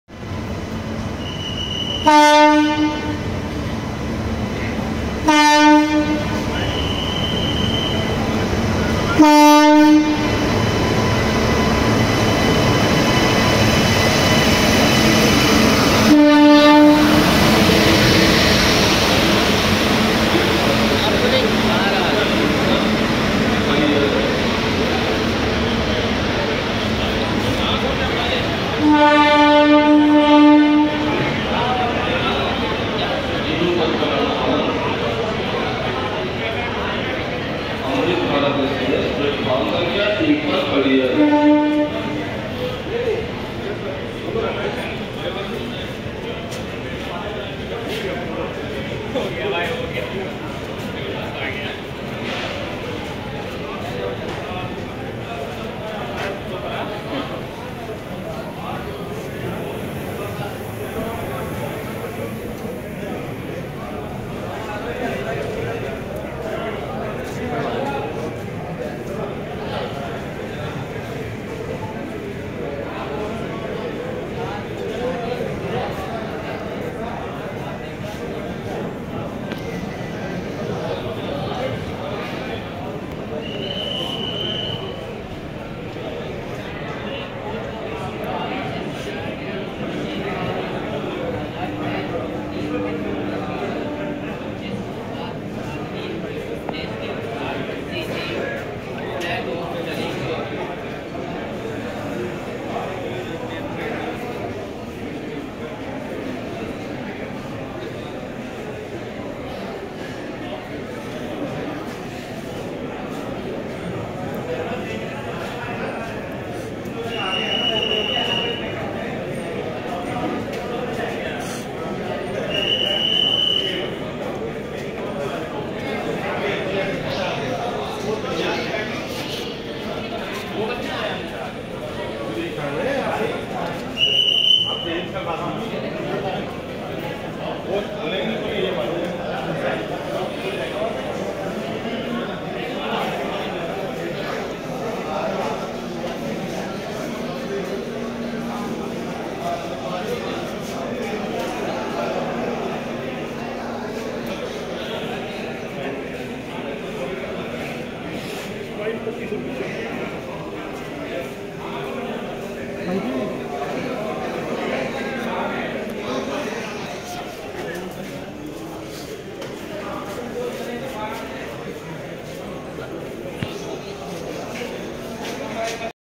First Inaugural Run Of 05558 Ayodhya Dham Jn Darbhanga Amrit Bharat Express Arriving Ayodhya Cant
05558 Ayodhya Dhan Junction Darbhanga Amrit Bharat Express Arriving Ayodhya Cant Railway Station